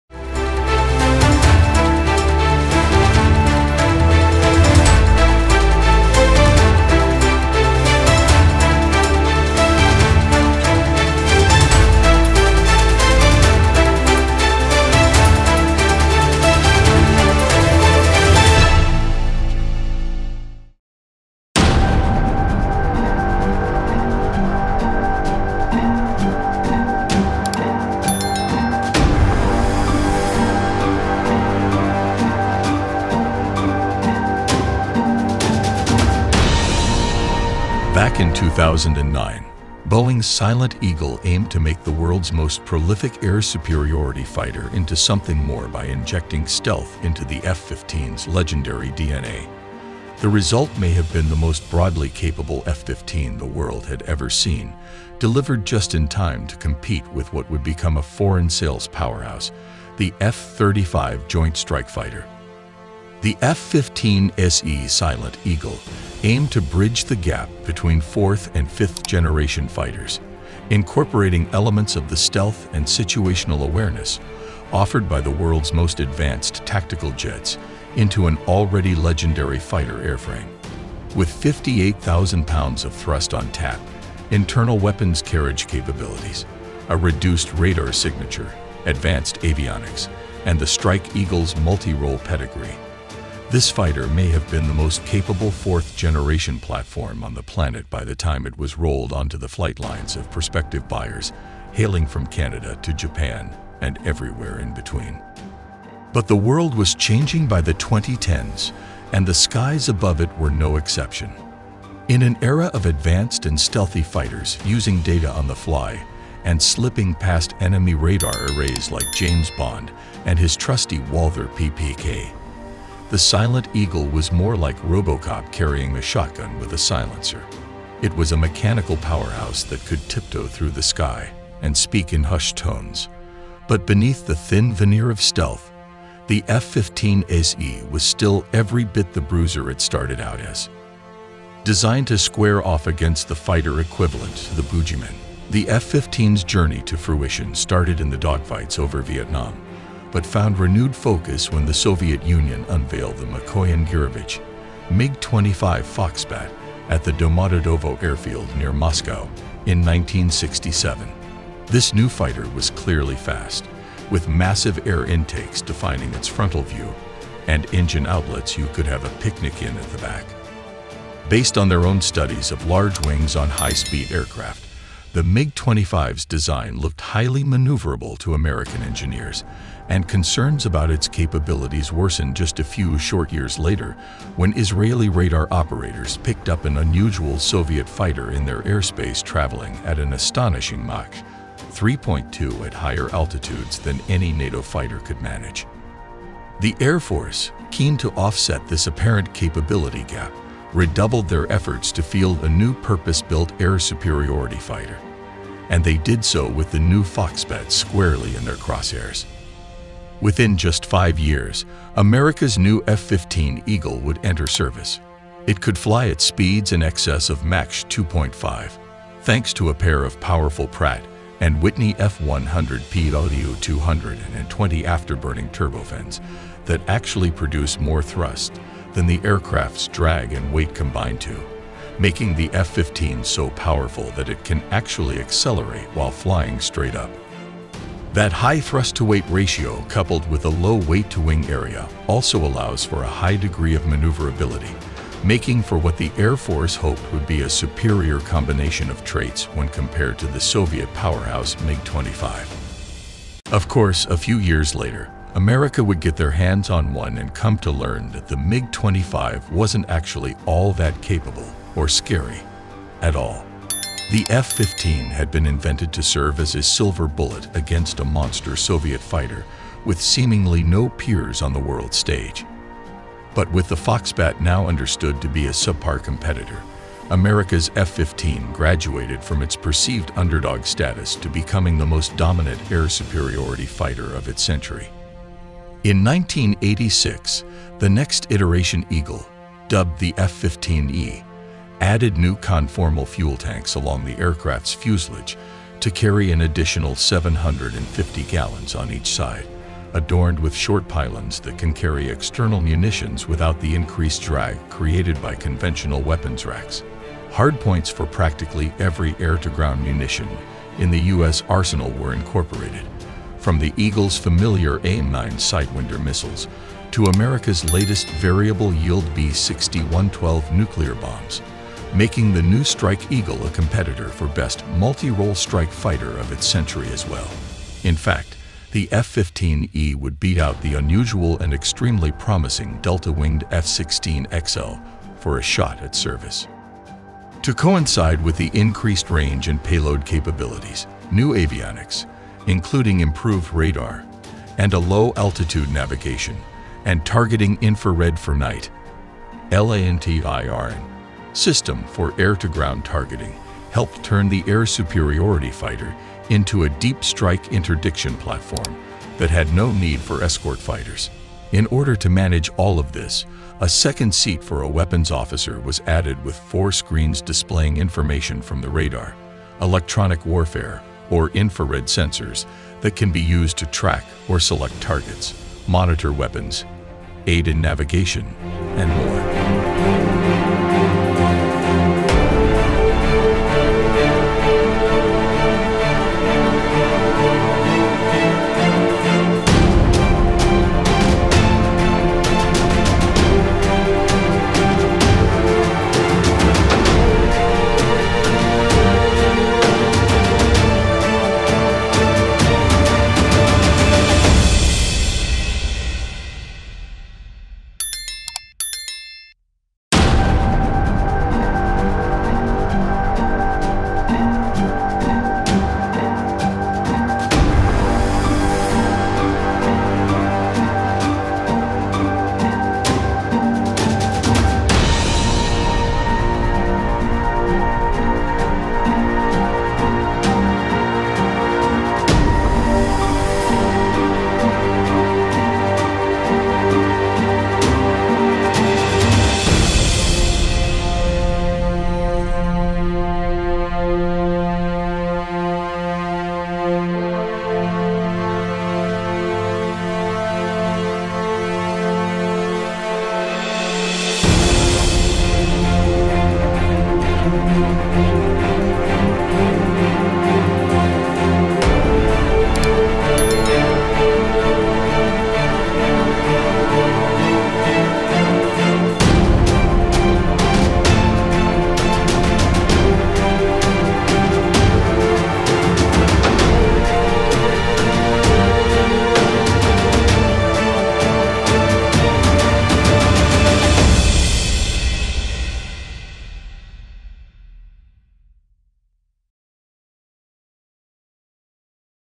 Shock: F-15 Silent Eagle Introduced As A High-Power Stealth Fighter
Today, the US Defense Department officially unveiled their newest high-power stealth fighter, the F-15 Silent Eagle. Capable of flying at over 2,000 mph, the F-15 Silent Eagle is a frightening weapon that will change the balance of power in the air.

If you're interested in the latest stealth technology, be sure to check out the F-15 Silent Eagle! This new fighter is a scary sight to behold, and it promises to change the balance of power in the air. Don't miss out on this incredible weapon!